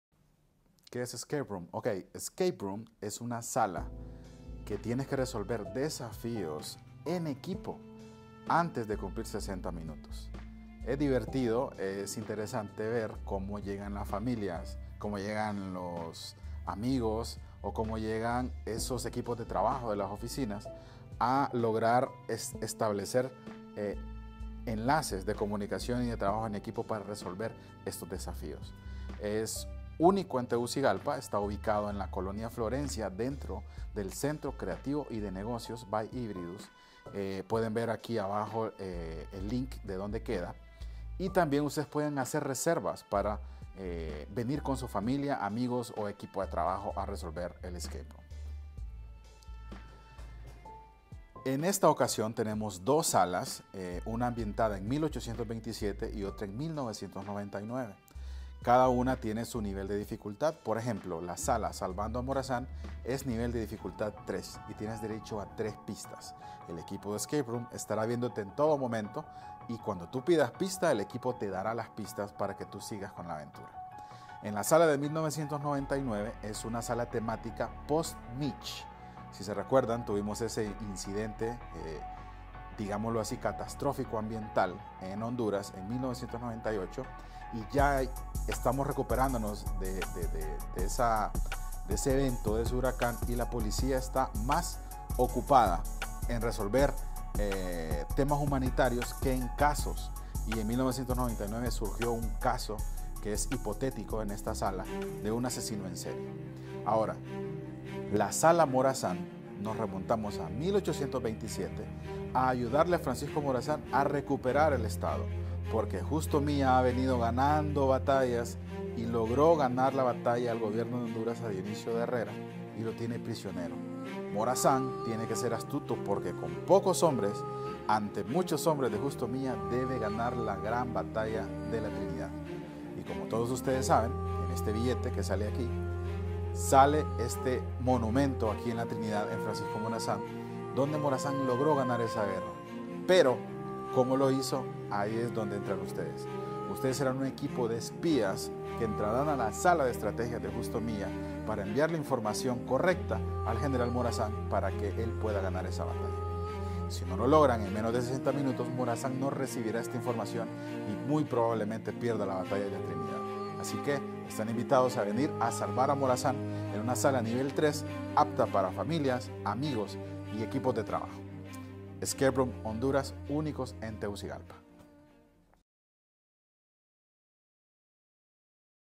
Conoce "Escape Room" donde tu ingenio y habilidades se unen para resolver desafíos
En la vibrante ciudad de Tegucigalpa, Honduras, una experiencia de entretenimiento sin igual espera cautivarte. Bienvenidos a Escape Room, donde la diversión, el ingenio y la colaboración se unen para desafiar tus habilidades y estimular tus sentidos.